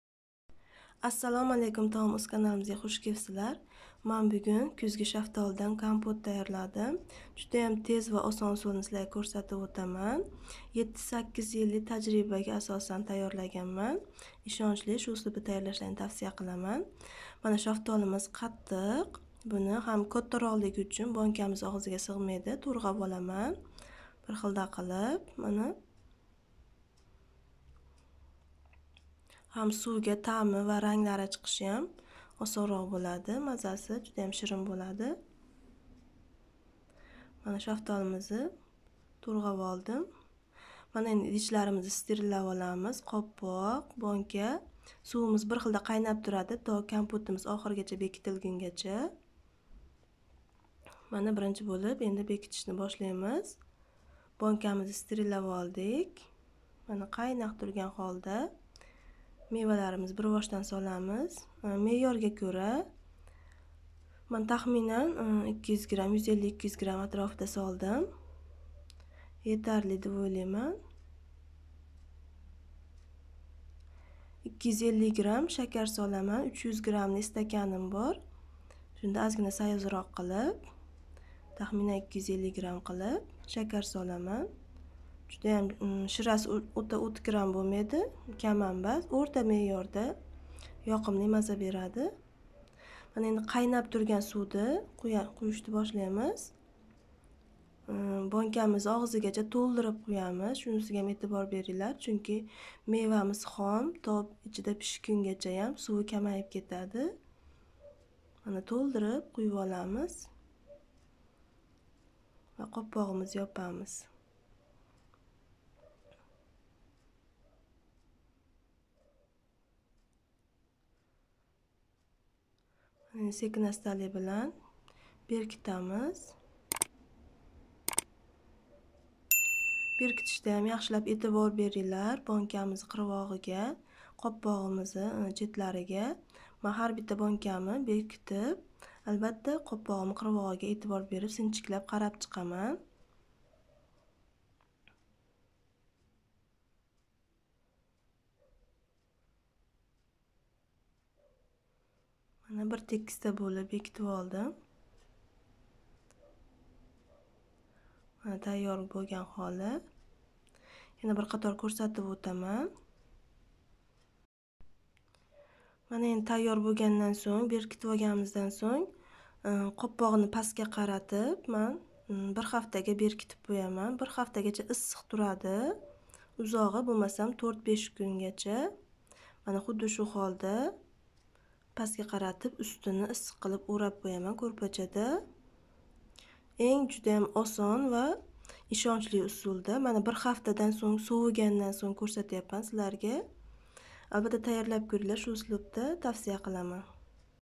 ШАФТОЛИ КАМПОТИ ТАЙЁРЛАШНИ ЕНГ ОНСОН ХАВФСИЗ УСУЛИ | Безопасный способ приготовить компот из персика
Ретсепт
Шафтоли 200 г
Шакар 250
Кайнаётган сув